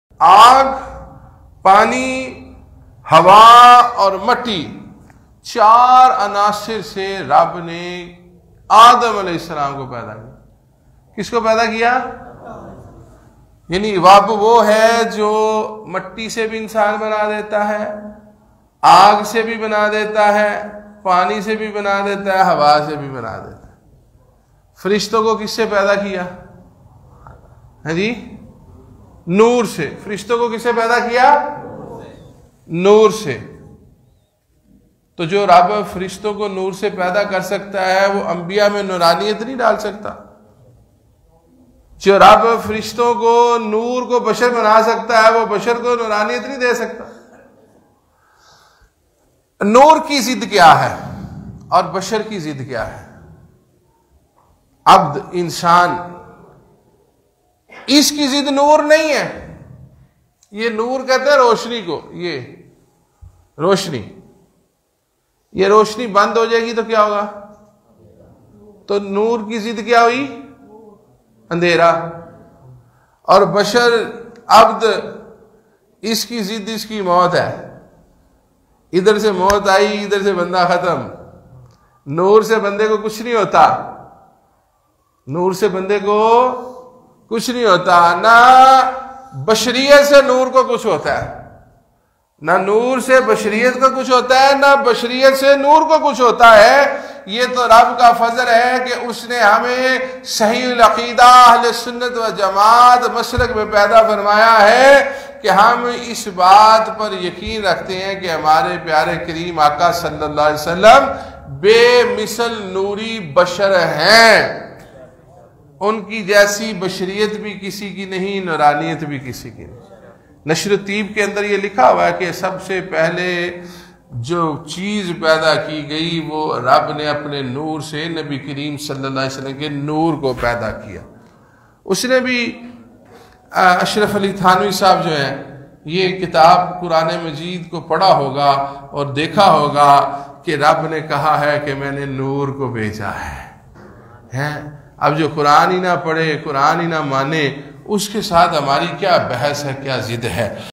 Noor o Bashar Ka Khas Hawala (Ilm Walo Ke Lie Ilmi Nuqta) nagina tv
Noor o Bashar Ka Khas Hawala (Ilm Walo Ke Lie Ilmi Nuqta) Peer e Tareeqat Hazrat Allama Sahibzada Bashir Ahmad Yousufi MCS